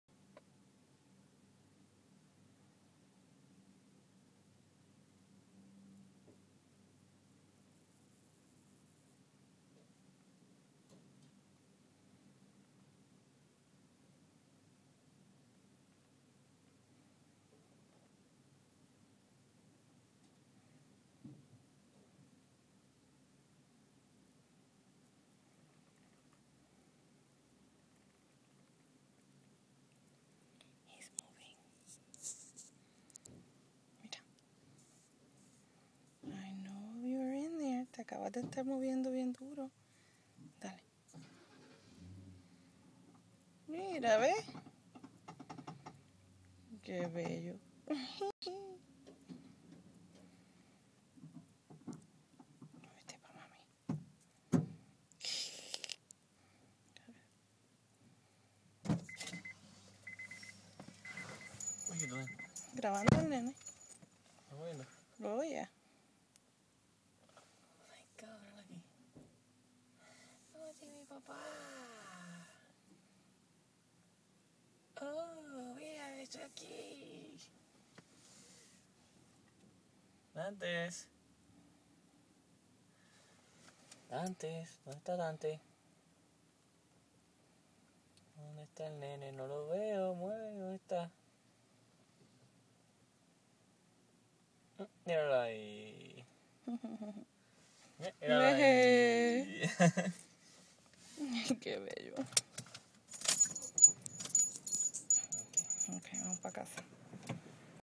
Pregnant belly 37 weeks
Baby moving in my 37 week pregnant belly. Watch my baby boy kick and roll around in there.